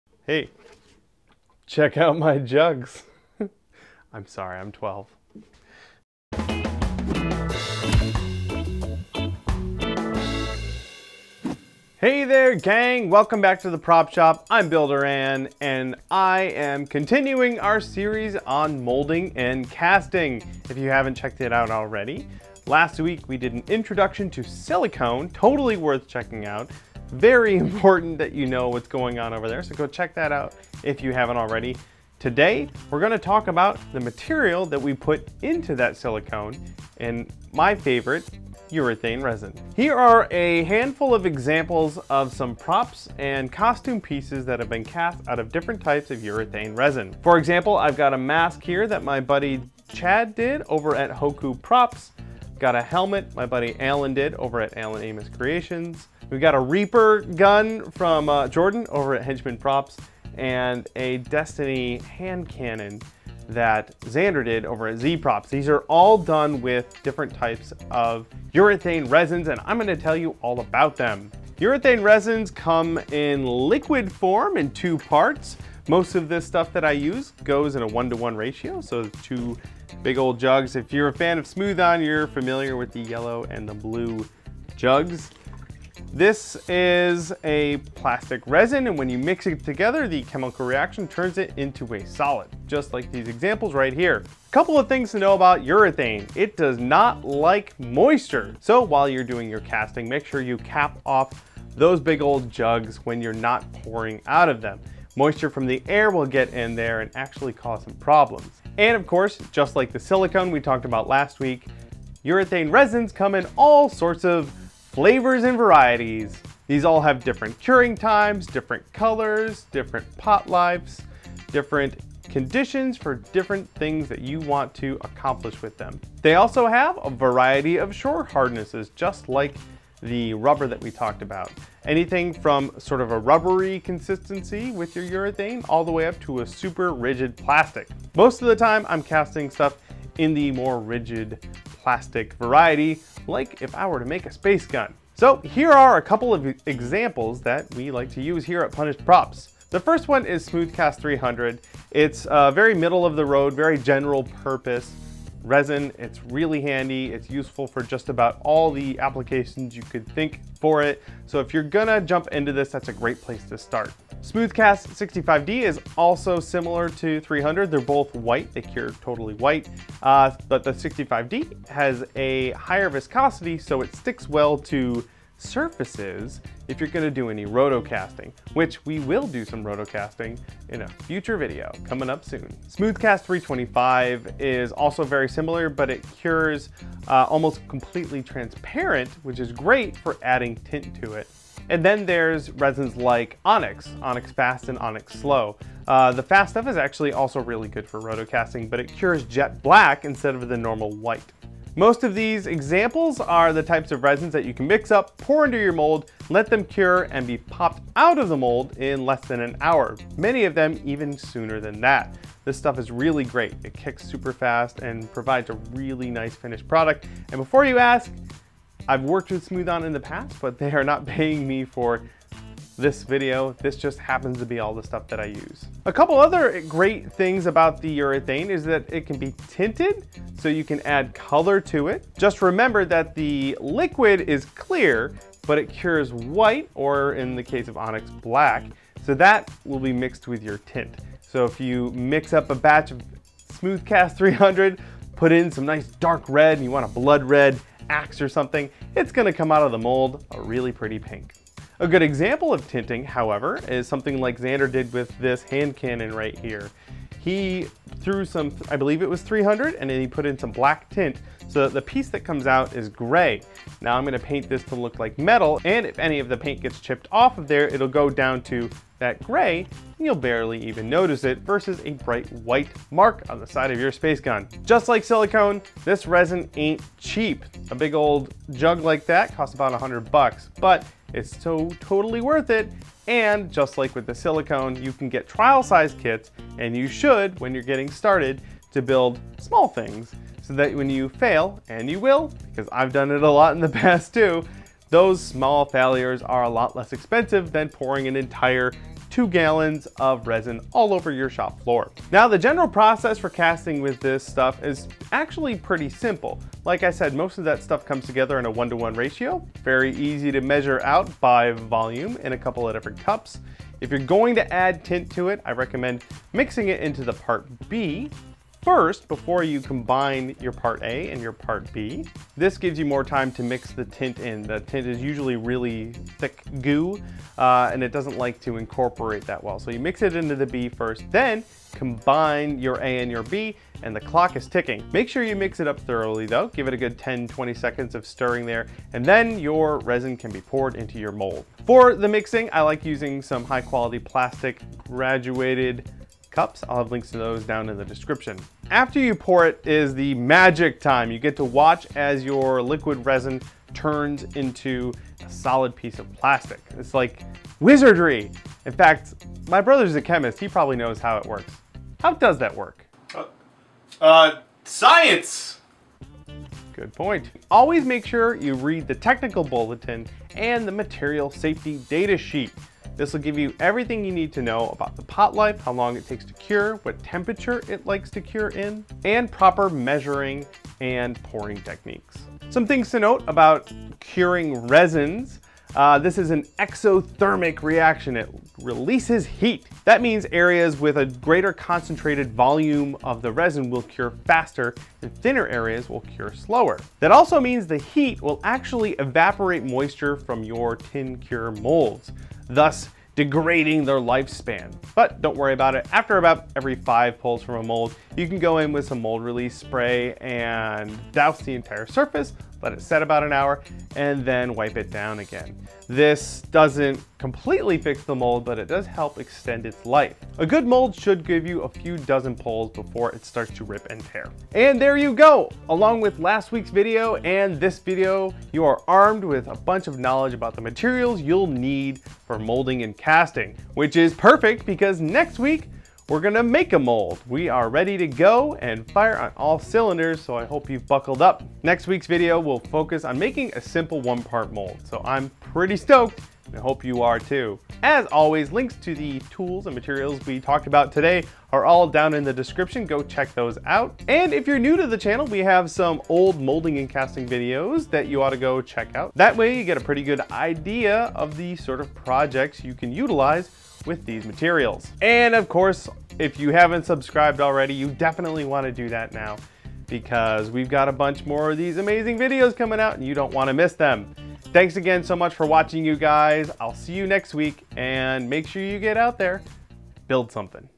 Prop: Shop - Molding & Casting 101: Intro to Urethane Resin
Everything you need to know about the urethane resin you'll be using to cast pieces in your silicone molds.

Materials:
Smooth-Cast 300

Smooth-Cast 65D

Smooth-Cast Onyx Fast

Smooth-Cast 325

Molds Release Spray

SO-Strong Tint

Resin Kits:
Allen Amis Creations

Hoku Props